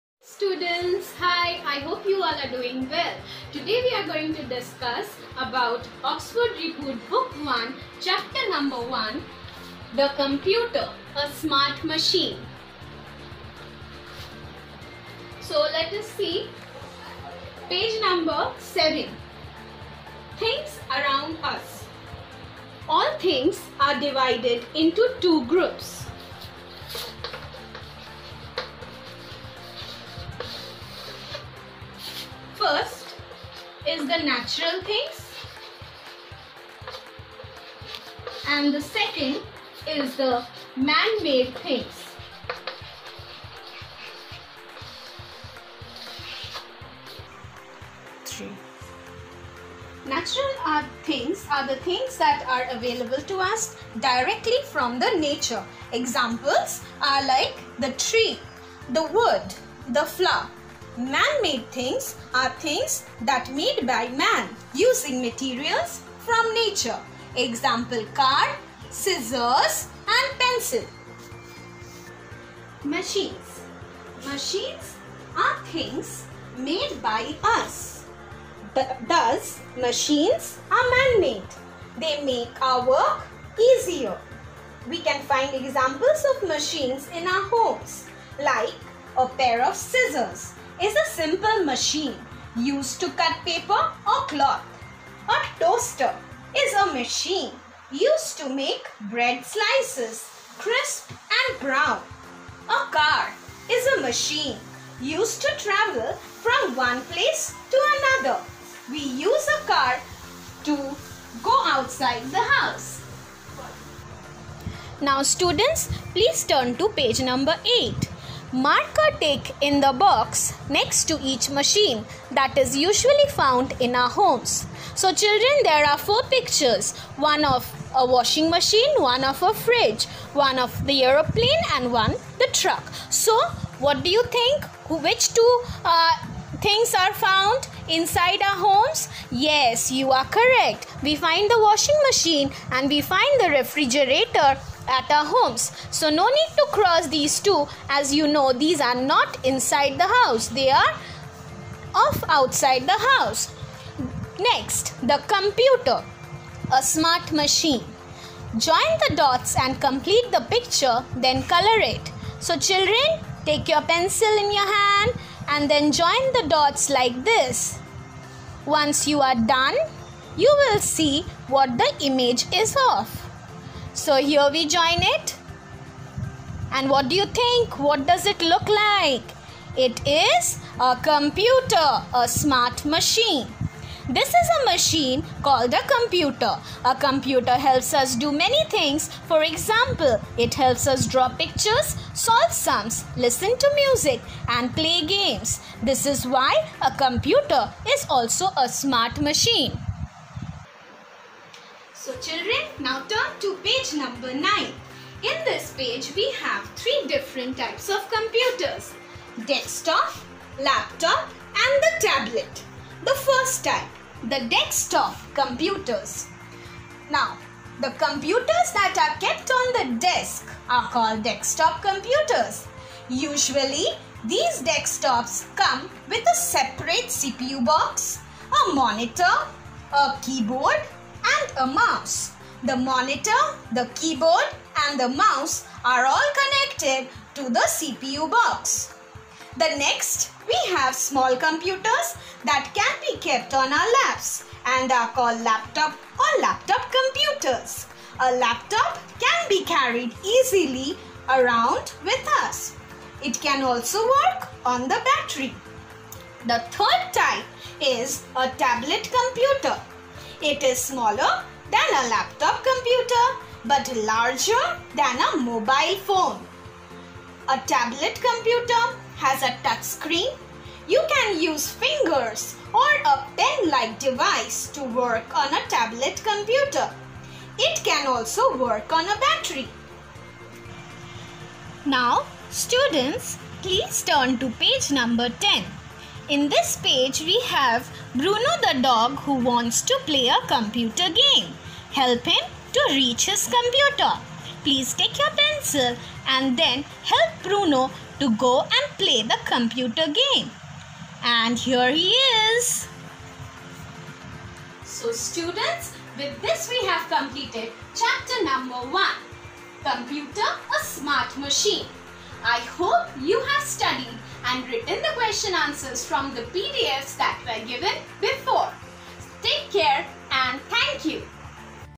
Computer class | Oxford Reboot Book 1 | Chapter 1 | The Computer- A Smart Machine| Std I
Hi there!
Due to the present situations due to the Covid 19 pandemic, and the schools remaining closed,  I have tried and made a video for the students to understand and read better.